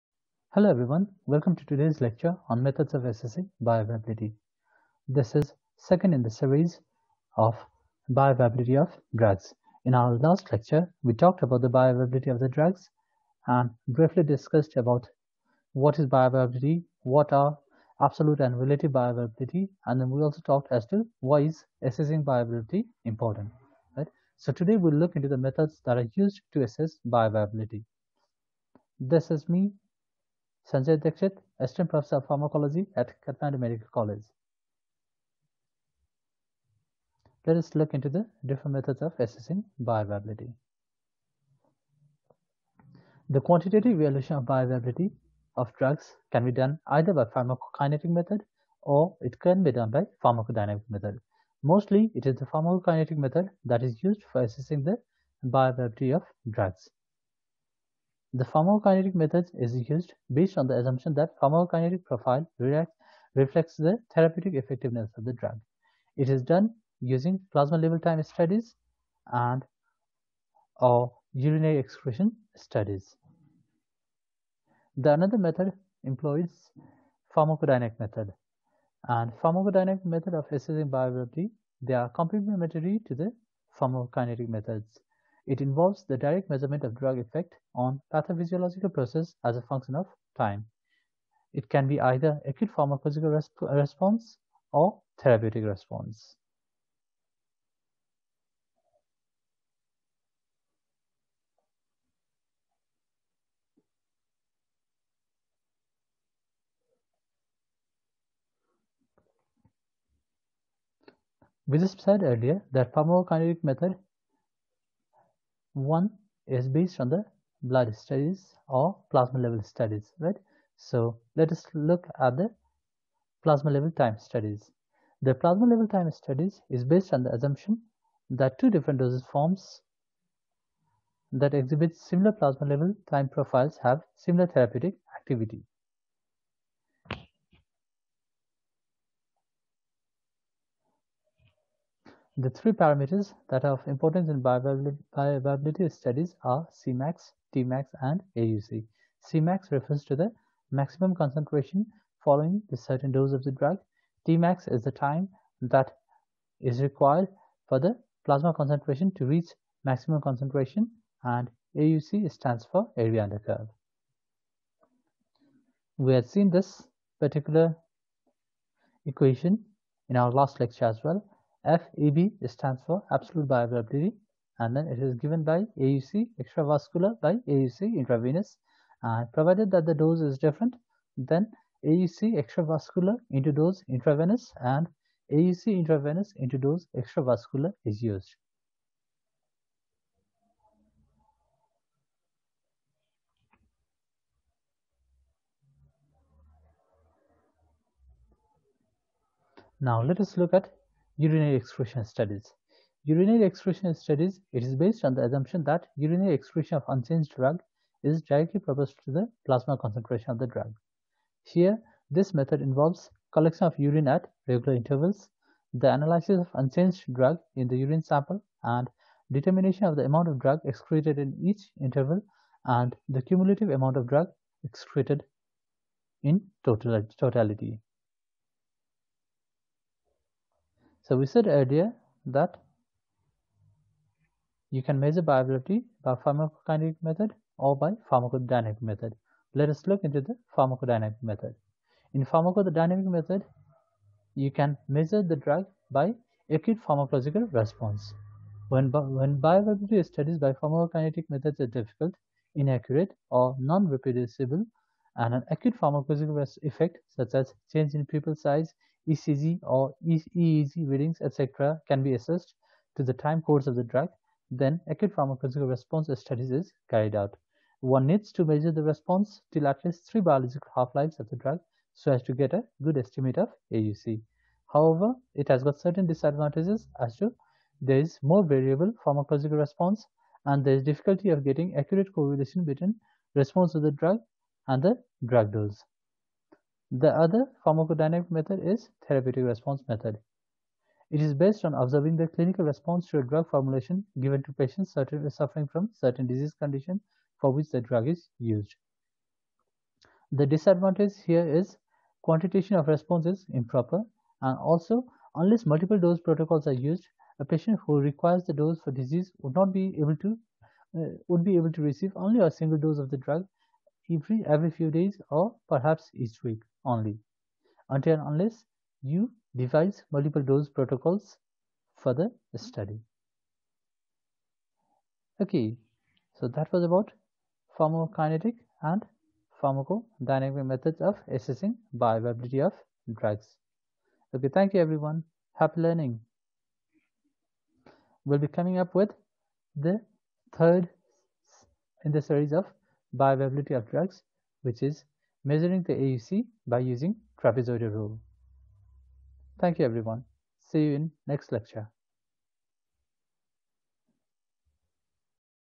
Methods of Assessing Bioavailability- BA of Drugs- Part 2 - Short Lecture Series on Pharmacology
In this informative lecture, we will explore the various methods used to assess the bioavailability of drugs. Understanding bioavailability is crucial for determining how effectively a drug reaches its intended site of action. This video will cover key assessment techniques, including the area under the curve (AUC) and other pharmacokinetic parameters that help evaluate drug absorption and effectiveness. Join us as we continue our Short Lecture Series on Pharmacology, designed to enhance your understanding of drug bioavailability and its significance in clinical practice.
Learn pharmacology as you play via game-based learning Medipuzzle App.